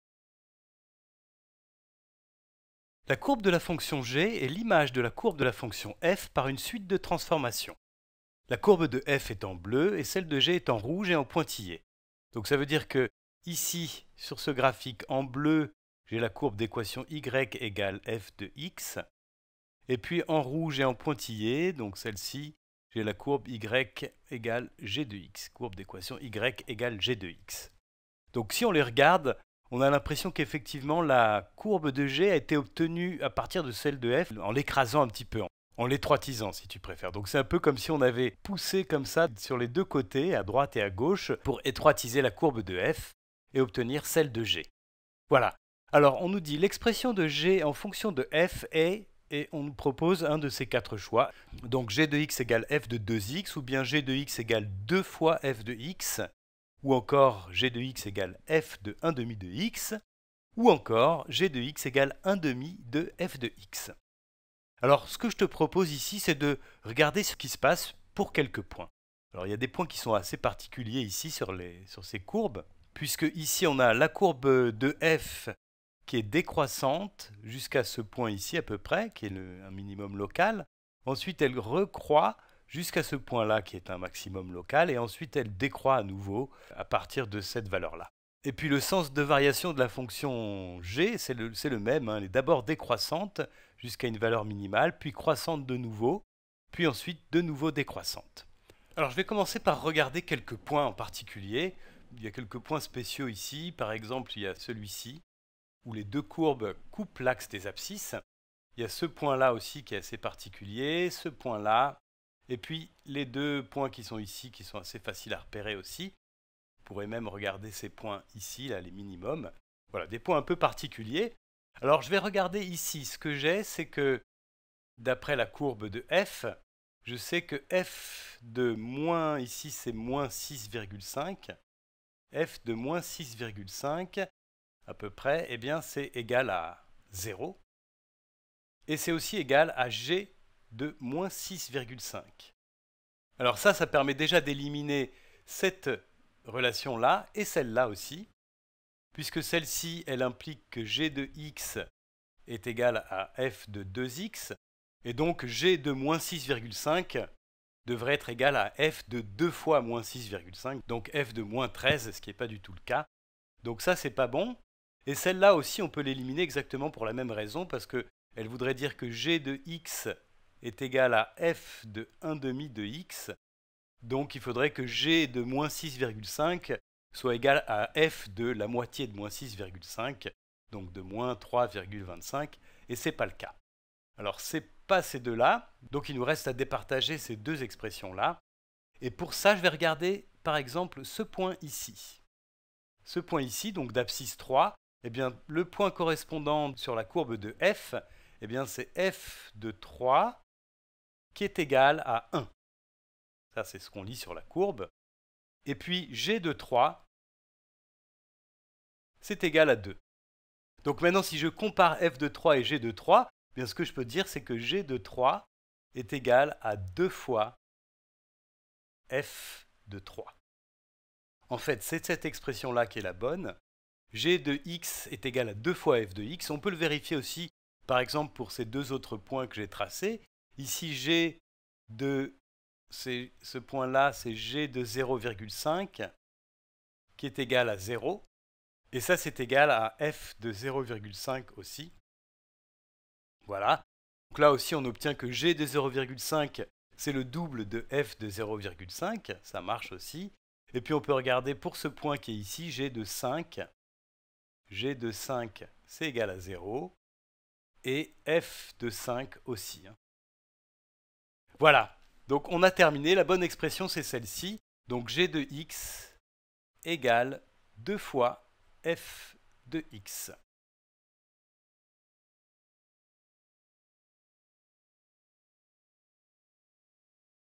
Expression d'une fonction définie à partir d'une autre par une suite de transformations
On détermine l'expression d'une fonction à partir de sa représentation graphique et de celle d'une autre fonction connue.